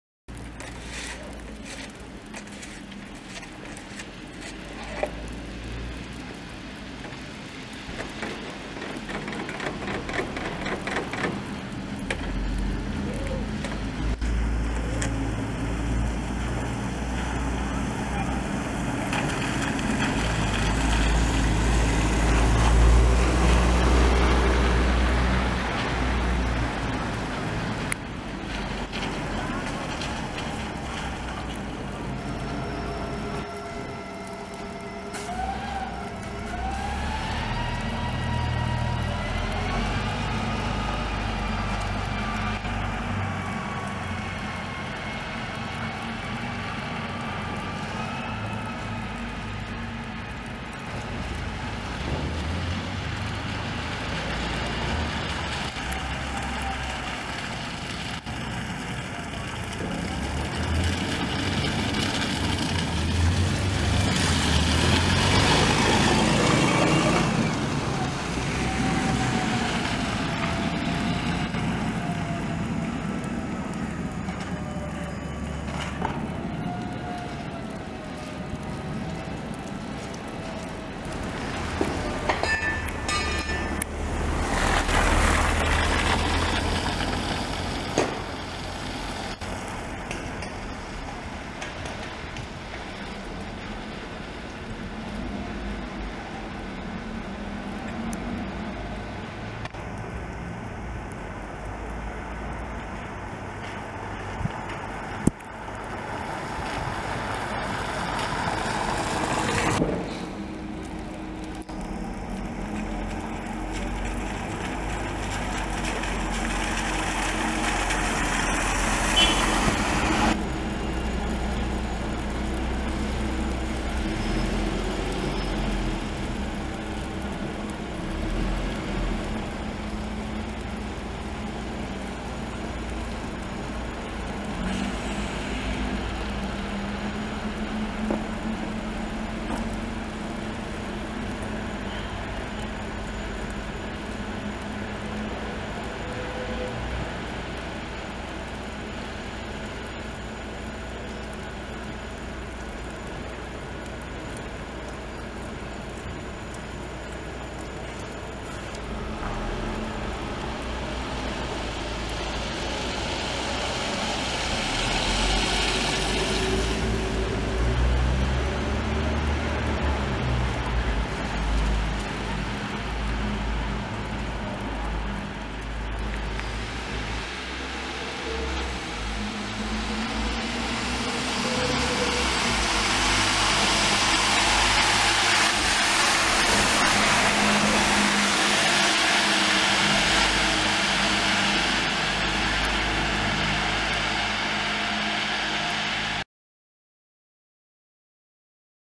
Buses in the snow - Vicenza (Italy)
Autobus SVT, in transito/fermata presso Piazza Castello, nel centro storico di Vicenza, sotto una fitta nevicata il giorno 13 dicembre 2019.
COPYRIGHT AlebusITALIA